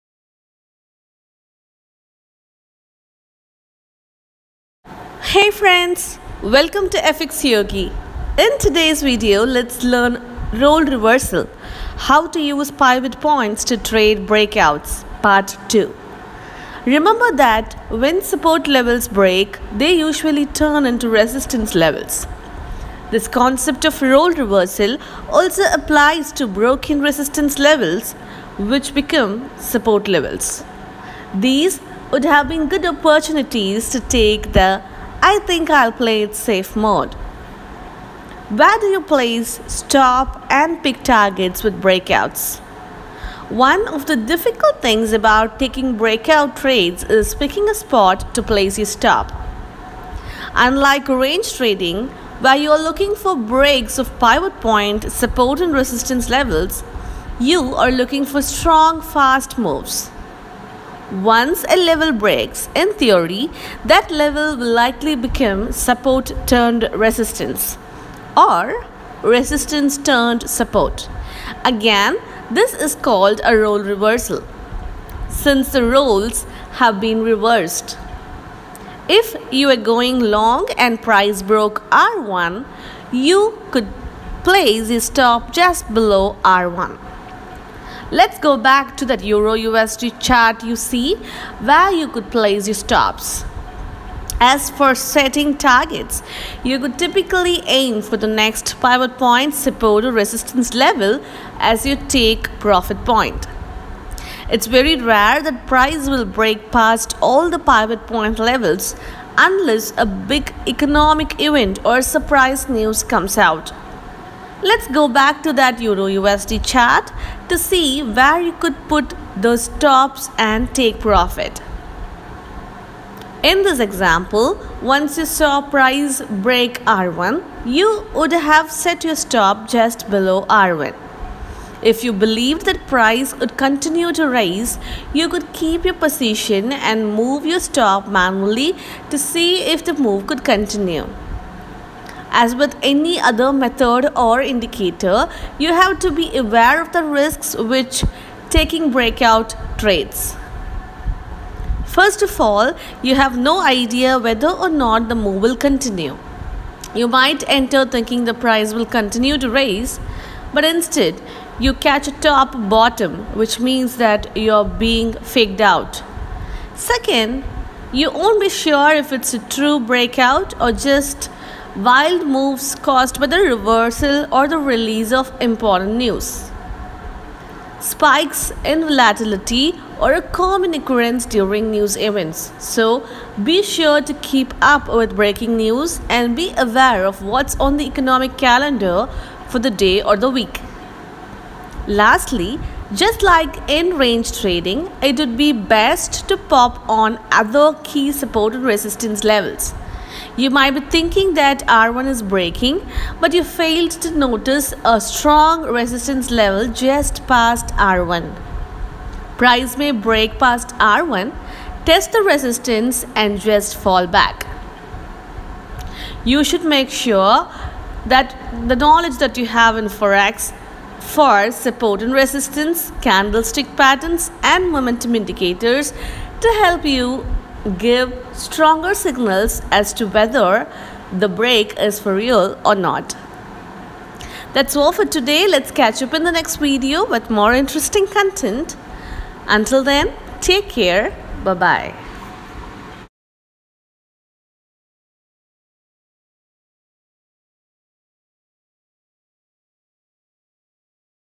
Role reversal how to use pivot points to trade breakouts
This concept of “role reversal” also applies to broken resistance levels which become support levels.